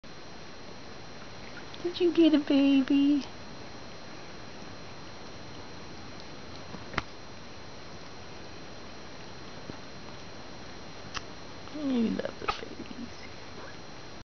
PampiLovesCassieAndApolloMVI_2085.AVI
Pampi is such a good foster mama! He loves Cassie and Apollo.